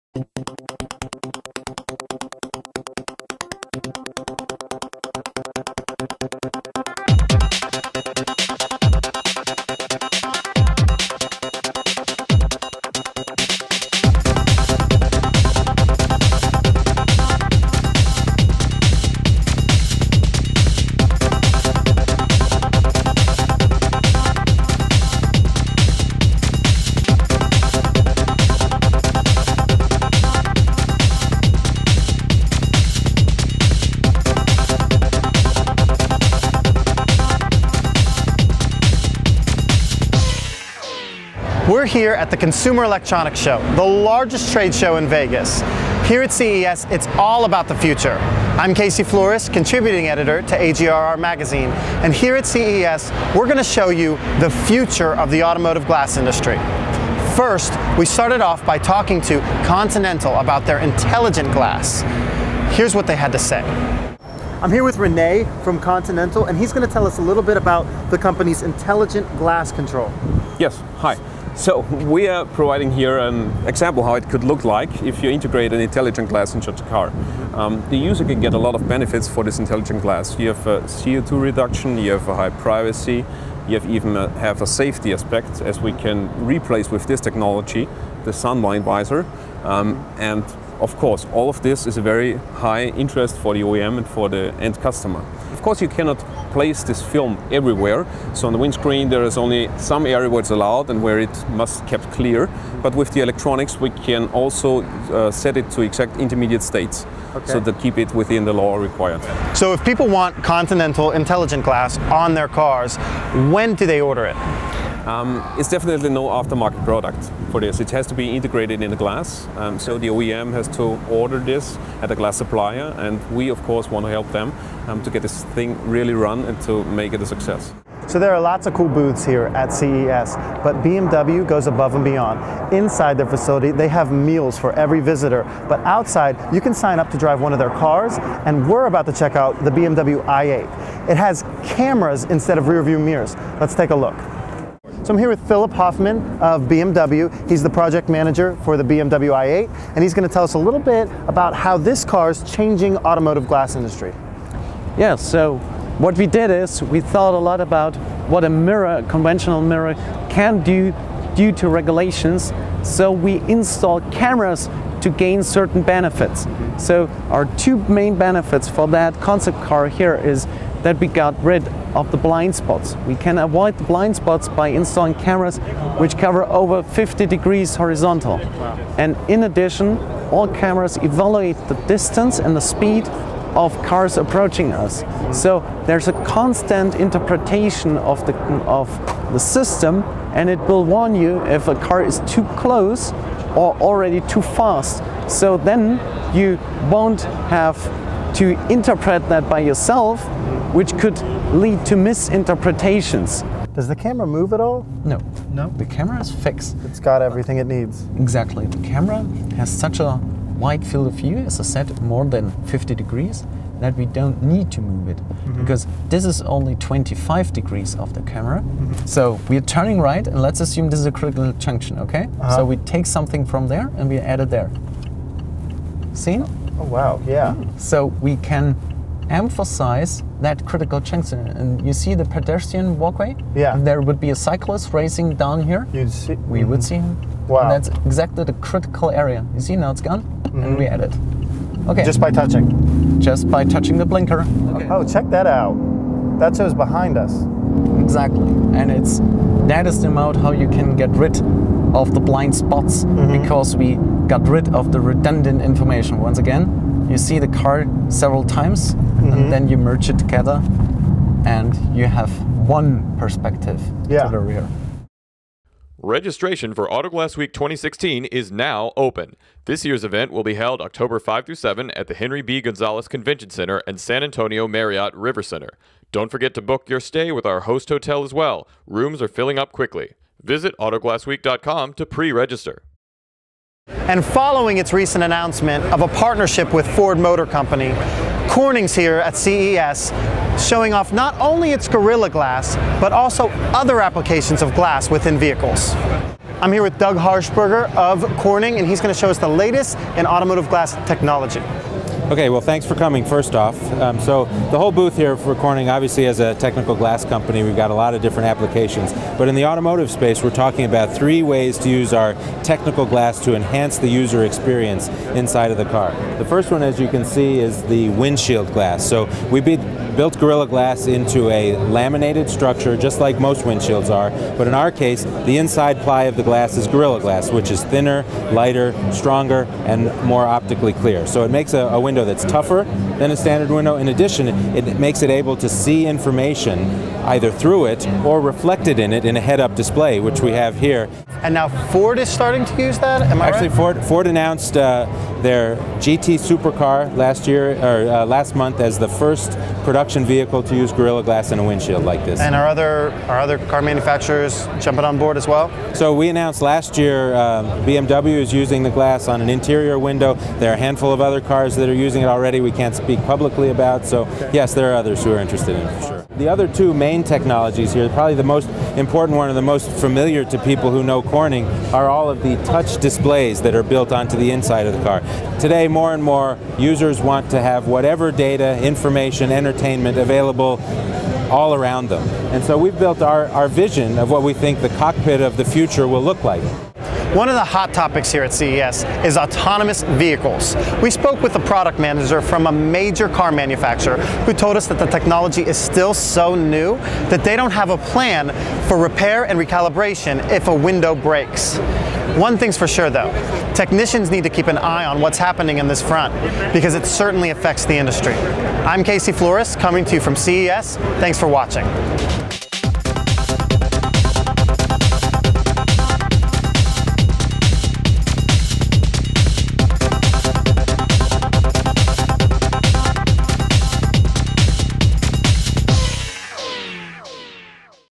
Newscast: The Future of Automotive Glass
Our editors go onsite to the top technology show—CES in Las Vegas—to bring you some of the latest developments dealing with automotive glass. Learn what Corning, BMW and others have in store for the industry.